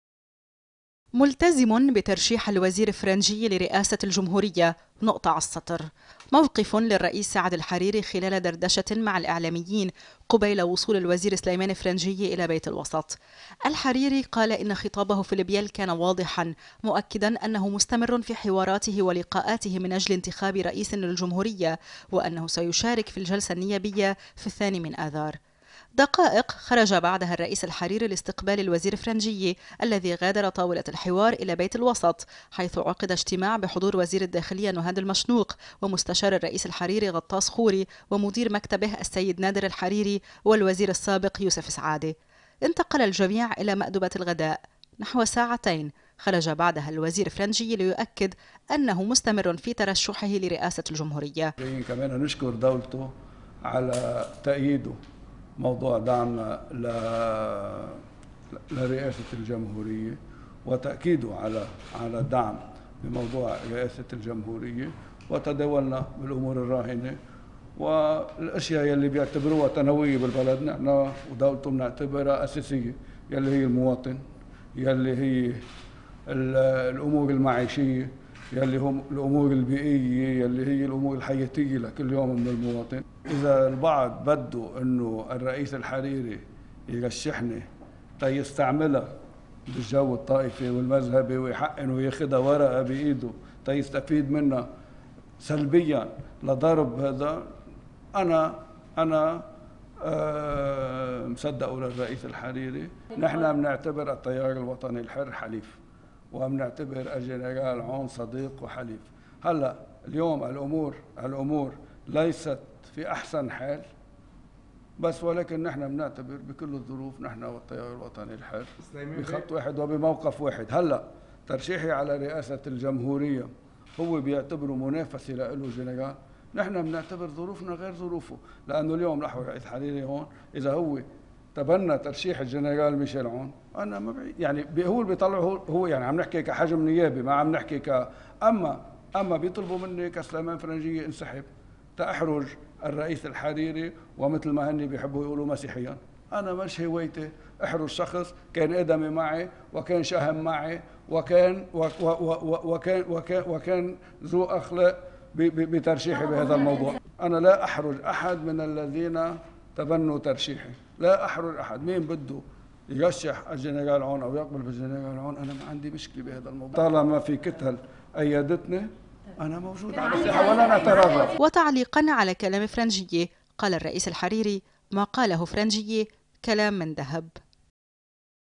فرنجية يؤكد من بيت الوسط استمراره في ترشّحه للرئاسة
زار المرشّح سليمان فرنجية بيت الوسط والتقى الرئيس سعد الحريري وجرى بحث الملفات الاساسية العالقة في البلاد وفي مقدمتها الملف الرئاسي.
تقرير: مهى ضاهر